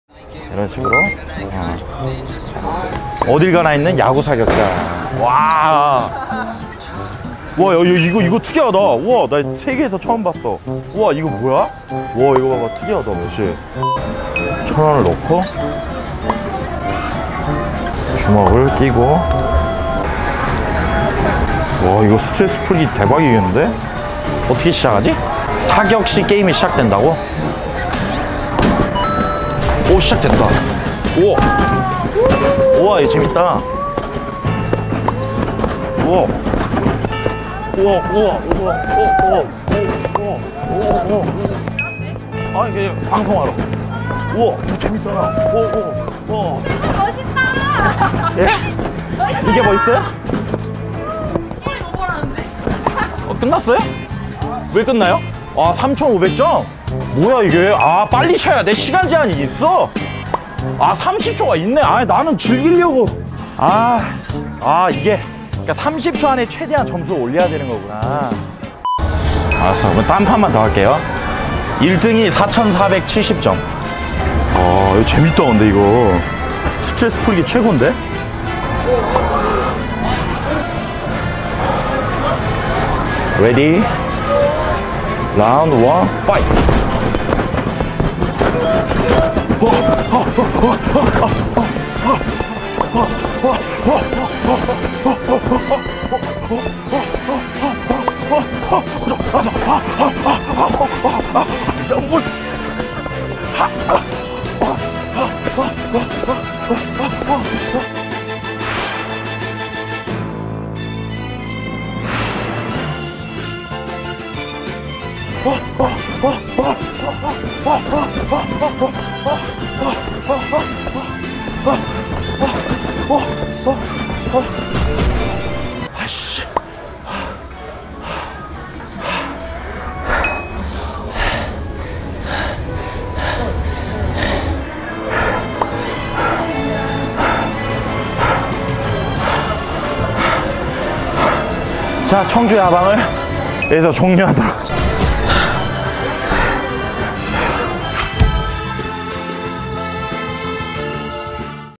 폭행? 청주 충대중문에서 시비가 붙어버린 최군 [oh Hot] - KoonTV
청주 충북대 중문을 방문한 최군!
지나가다가 신기한 오락기를 발견하는데.. 권투? 과연 1등할 수 있을까?